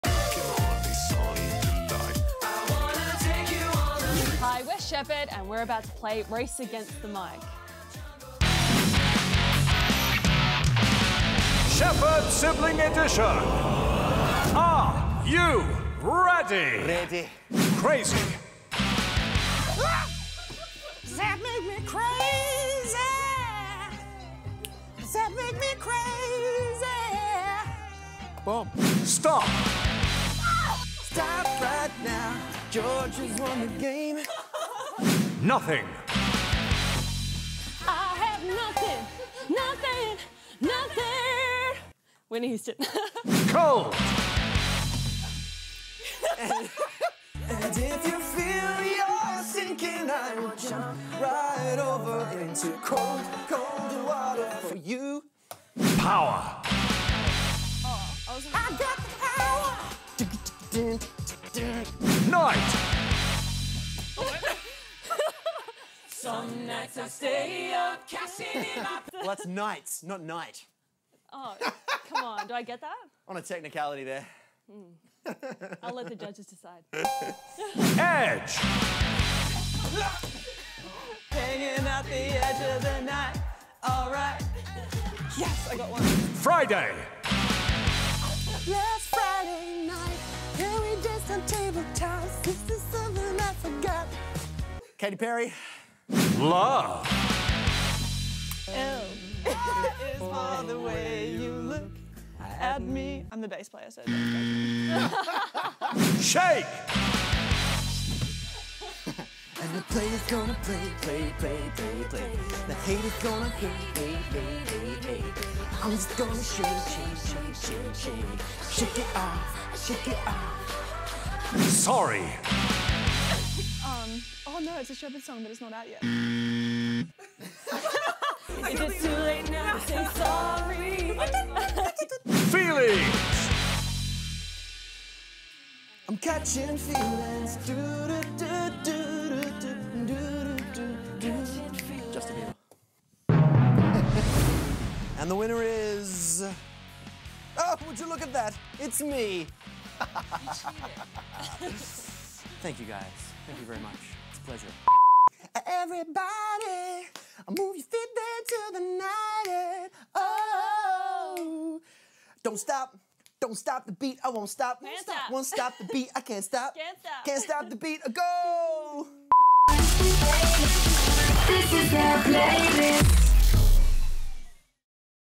Sheppard Play Race Against the Mic! | The Playlist
It's the Sheppard sibling edition of Race Against the Mic! Can you guess what song they'll sing?